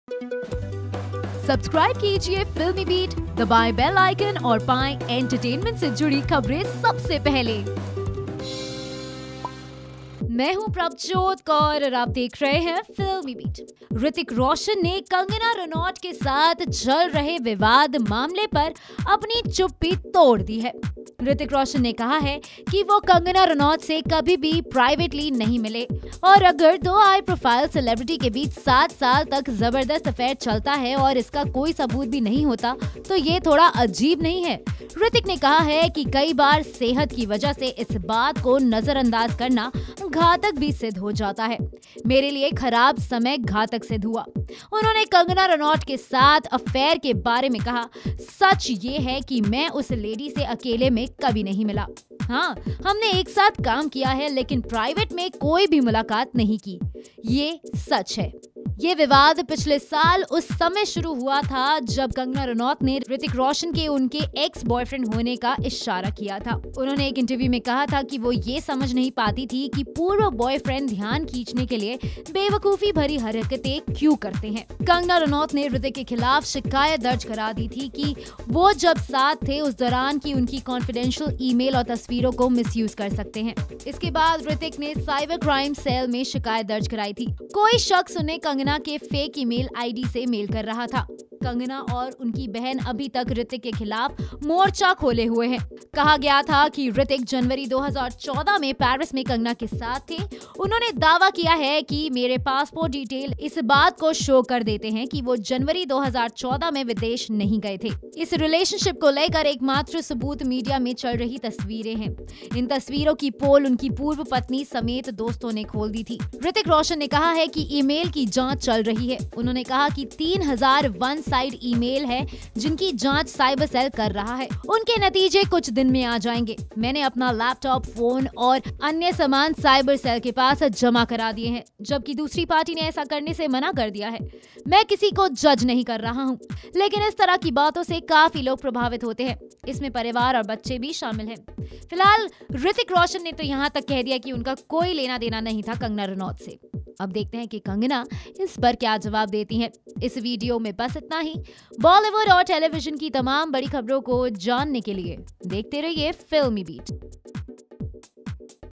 Hrithik Roshan REACTS for FIRST TIME on Kangana Ranaut; Here's how | FilmiBeat
Hrithik Roshan opens up with a two page letter on his equation with Kangana Ranaut and with all the controversies gathering around between him and Kangana. Watch video to know more.